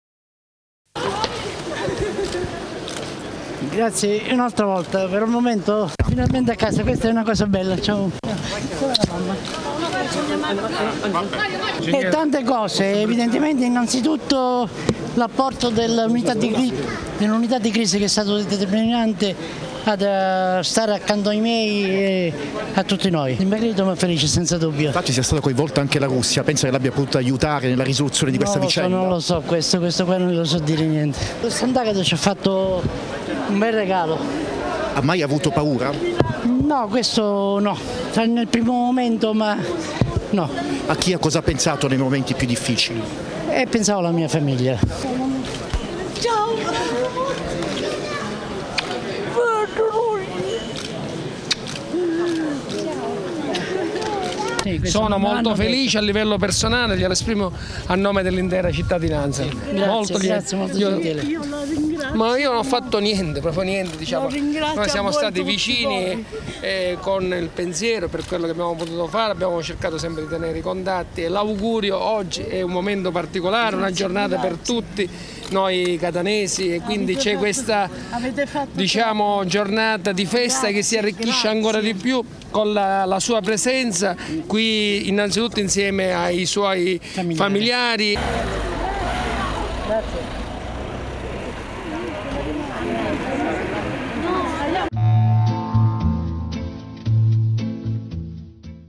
Belluomo torna a Catania, l'ingegnere rapito in Siria
"E' la vittoria dello Stato. Sono felicissimo di tornare a casa, Sant'Agata mi ha fatto un bel regalo". Lo ha detto l'ingegner Mario Belluomo, al suo arrivo all'aeroporto di Catania nel giorno dopo la sua liberazione dopo il sequestro del 12 dicembre scorso in Siria. "Sono grato alla Farnesina e all'unita' di crisi del ministero degli Esteri -ha aggiunto- per tutto quello che hanno fatto e per come sono stati vicini alla mia famiglia". "I sequestratori -ha evidenziato l'ingegnere- mi hanno trattato bene e mi hanno informato della mia liberazione il giorno stesso del rilascio".
Servizio di Salvo Falcone
Editing di Guido Messina